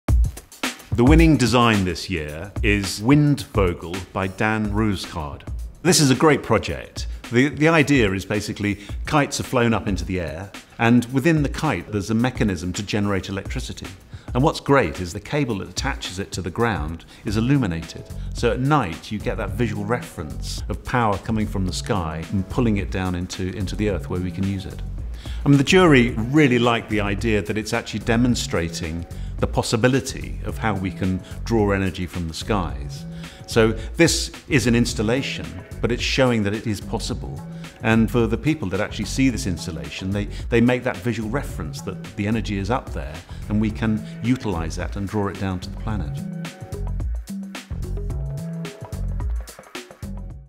Windvogel by Studio Roosegaarde: Design Project of the Year | Dezeen Awards
The Dezeen Awards jury named Windvogel by Studio Roosegaarde as Design Project of the Year for the way the light-emitting kites draw attention to a new renewable energy source, says jury chair Paul Priestman in this movie.

Windvogel by Studio Roosegaarde is a lighting installation comprised of a pair of energy-generating kites tethered to a dynamo by glowing cables.

The project won the award for Lighting Design of the Year at the Dezeen Awards ceremony in London last week, as well as receiving one of the top honours of the night by being crowned Design Project of the Year 2018.

British designer Paul Priestman, who was the chair of the Dezeen Awards design master jury, says the project won because of the simple way it showcases how kites could be used to generate renewable energy.

"The jury really liked the idea that Windvogel demonstrates the possibility of how we can draw energy from the skies," he explains in the movie, which Dezeen filmed at the judging day in London earlier this year.

"This is an installation, but it's showing what is possible."